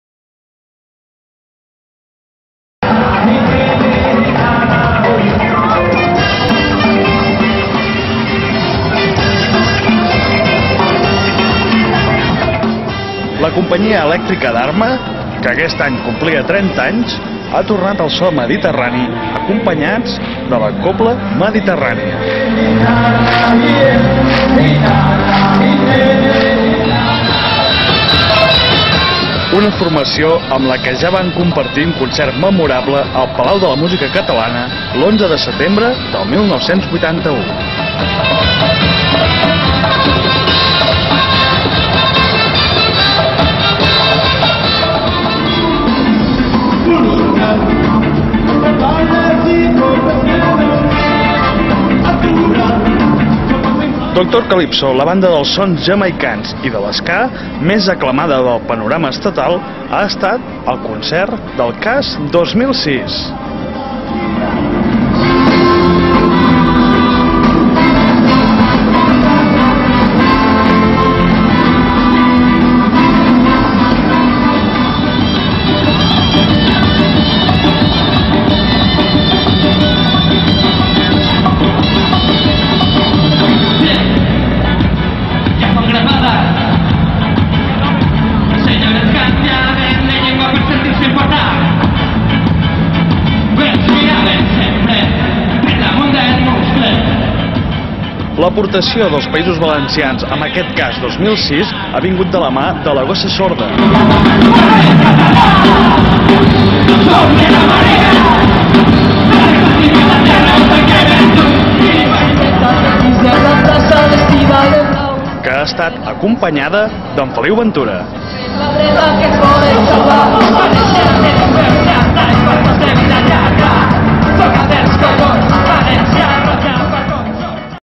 La gossa sorda (22-4-2006)
StockdesoTV.  La gossa sorda en directe des del Palau Sant Jordi de Barcelona, el 22 d'abril de 2006.